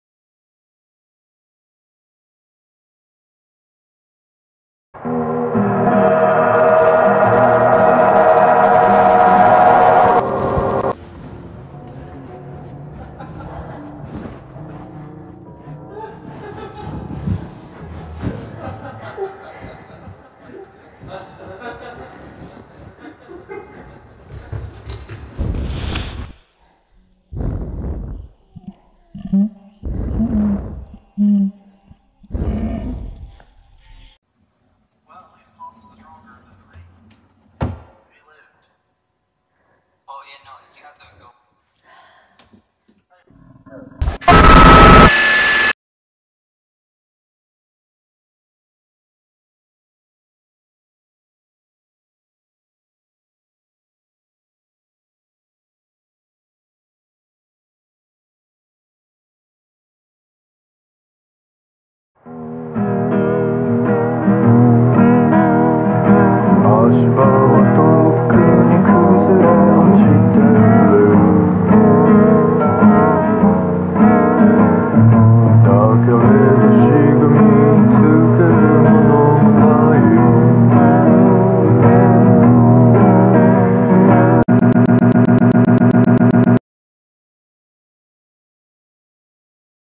"This Isn't My Boyfriend" - ANALOG HORROR [Re-Edit]
Me and Liz worked on a new possible Series of Analog Horror and hope you enjoy.